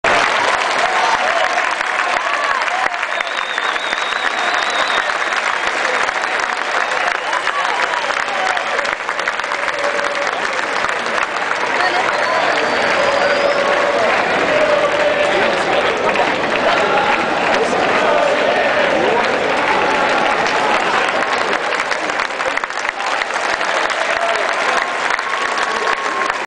Sale Ferro a la cancha
19° Fecha - Ferro 1 - Independiente Riv. 1

Salida del equipo